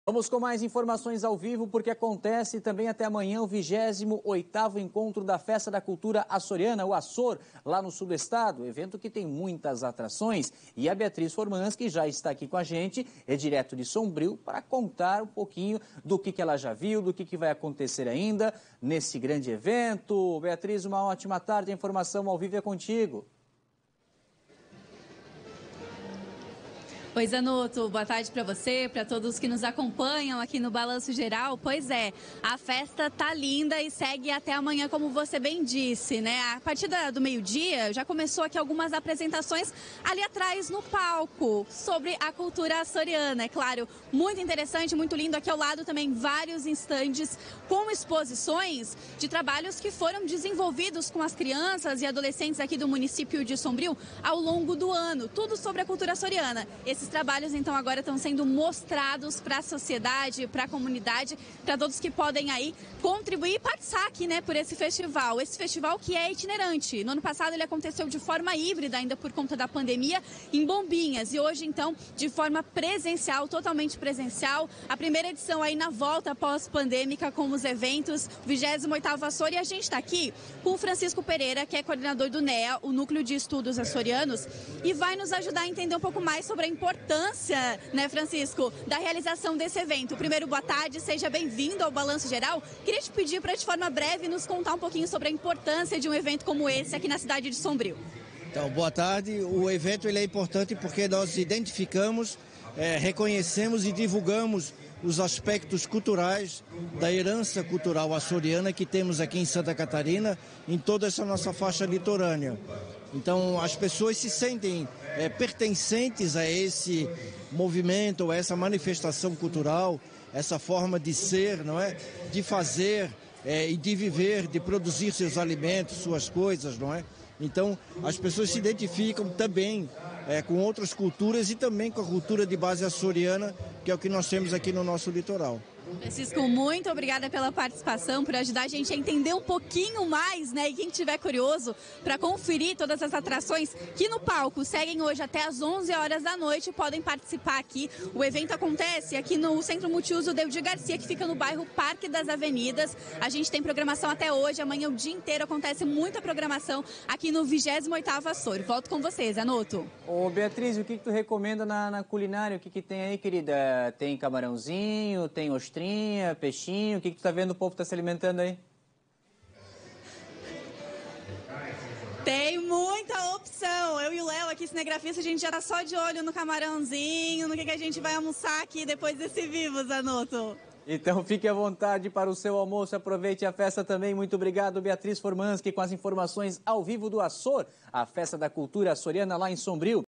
28ª edição da Festa da Cultura Açoriana acontece até domingo, em Sombrio
Acompanhe os detalhes da 38ª Açor, com a repórter Beatriz Formanski.